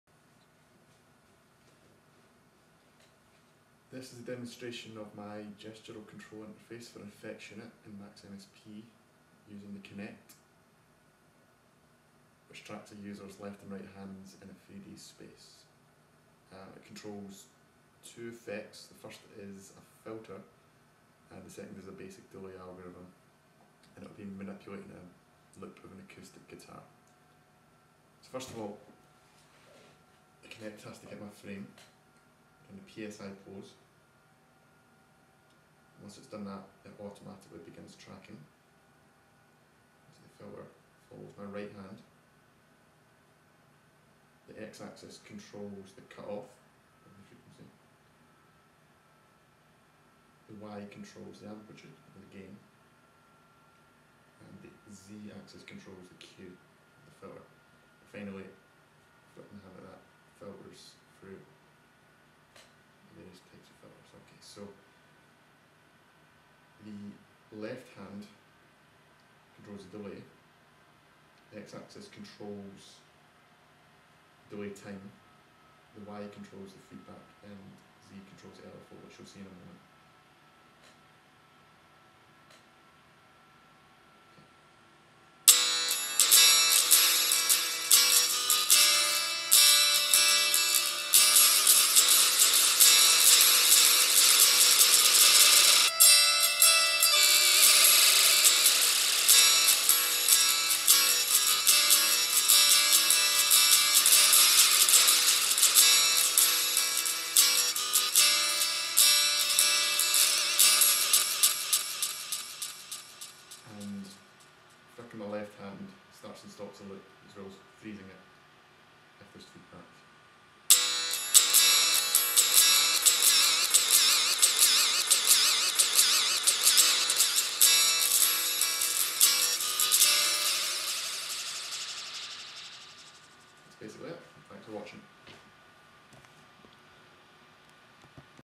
Kinect Audio Manipulation
The use of gestural interaction in any creatively orientated software has been in the epicenter of current research as an alternative and more natural way of interaction with software and hardware. Technological advances in computing offer a plethora of alternative visual, audio and gesture interfaces. Specifically in music composition, the software packages available currently utilise graphical user interfaces (GUI) to interact with the musician. This method of interaction is in stark contrast to the way in which a musician interacts with a musical instrument, which typically involves significant tactile and gesture interactivity. In this work we investigate a replacement to conventional graphical user interfaces in the form of a gestural interface.  Project by Colin Kirk.